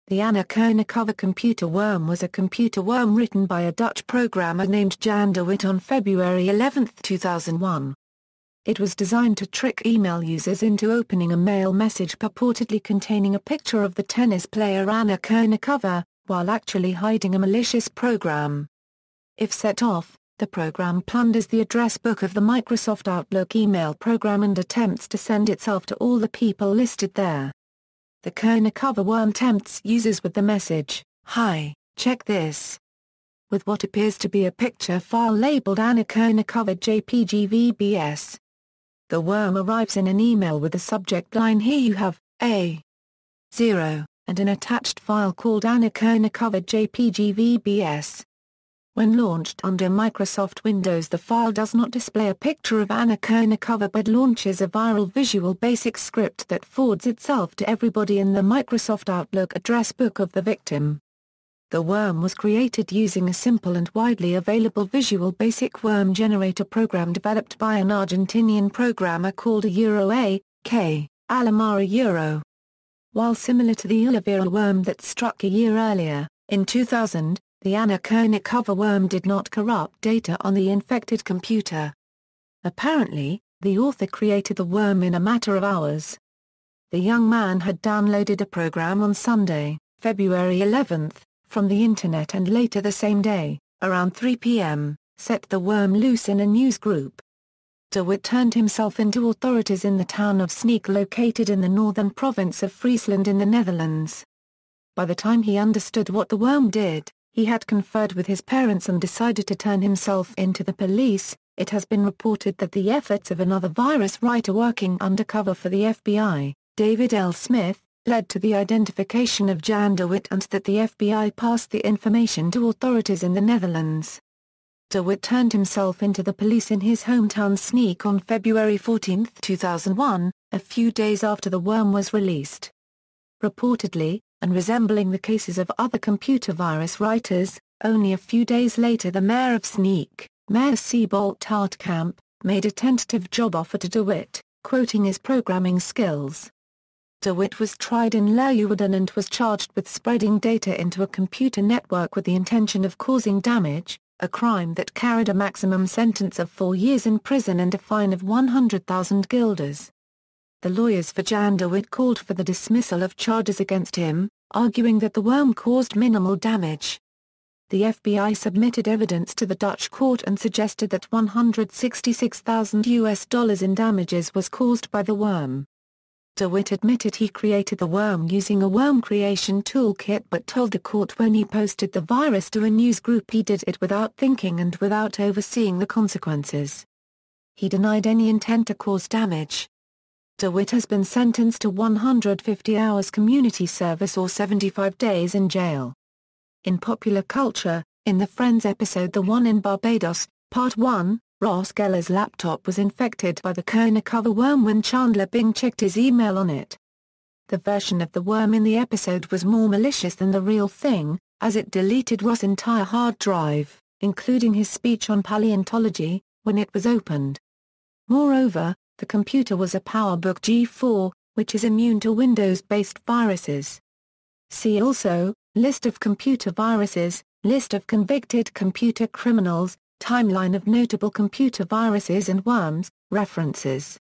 Anna Kournikova (computer virus)
The Anna Kournikova computer worm was a computer worm written by a Dutch programmer named Jan de Wit on February 11, 2001. It was designed to trick email users into opening a mail message purportedly containing a picture of the tennis player Anna Kournikova, while actually hiding a malicious program. If set off, the program plunders the address book of the Microsoft Outlook e-mail program and attempts to send itself to all the people listed there. The Kournikova worm tempts users with the message: "Hi: Check This!", with what appears to be a picture file labelled "AnnaKournikova.jpg.vbs". The worm arrives in an email with the subject line "Here you have, ;0)" and an attached file called AnnaKournikova.jpg.vbs. When launched under Microsoft Windows the file does not display a picture of Anna Kournikova but launches a viral Visual Basic Script that forwards itself to everybody in the Microsoft Outlook address book of the victim.
The worm was created using a simple and widely available Visual Basic Worm Generator program developed by an Argentinian programmer called “[K]Alamar”. While similar to the ILOVEYOU worm that struck a year earlier, in 2000, the Anna Kournikova worm did not corrupt data on the infected computer.